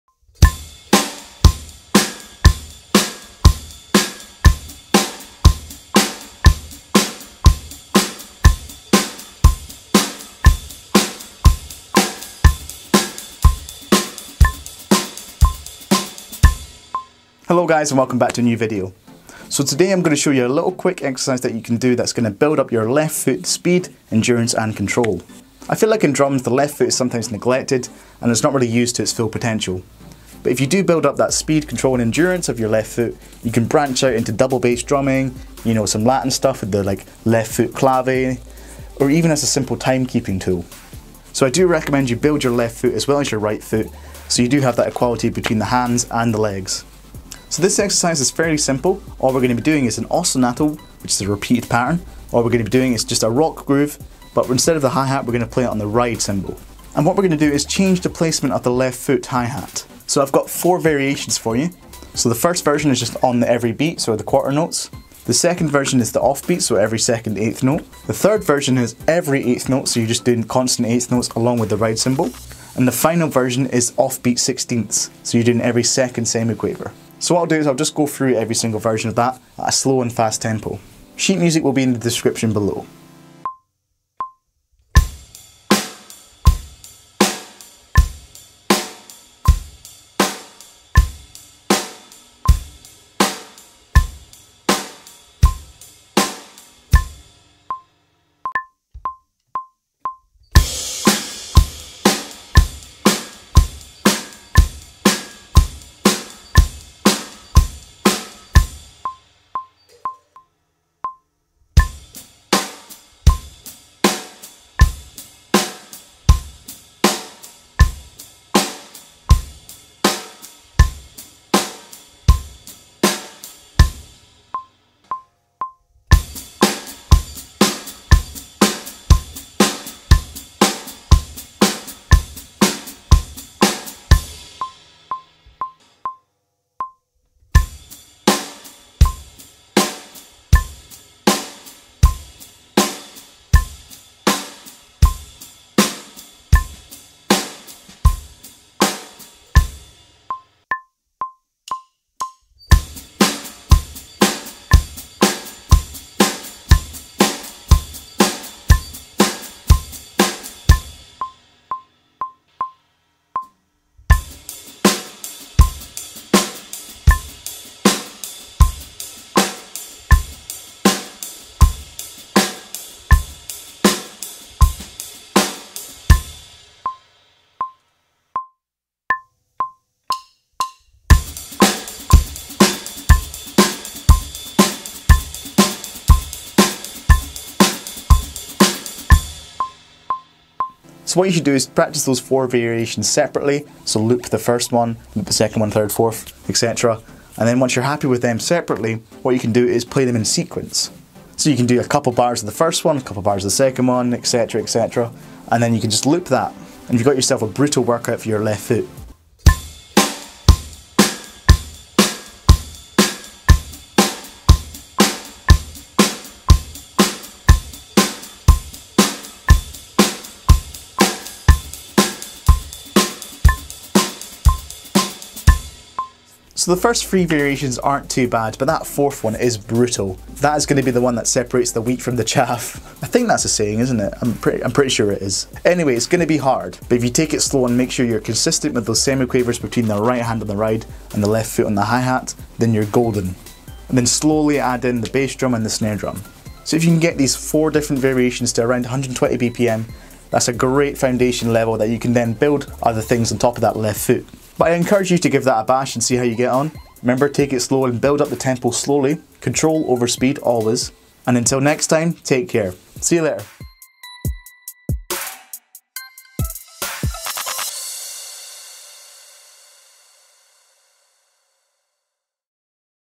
The Left Foot Workout - Free Drum Lesson | Rohan Bumbra
Hello and welcome to a new free drum lesson! Today we are looking at building our left foot speed, control and endurance.

It's a great exercise for beginners and pros alike, and is a great way to build up your left foot to a point where it is at the same strength as the lead foot!

This will give you the foundation to branch out into double bass drumming, Latin drumming, and timekeeping!

I hope you enjoy this little lesson!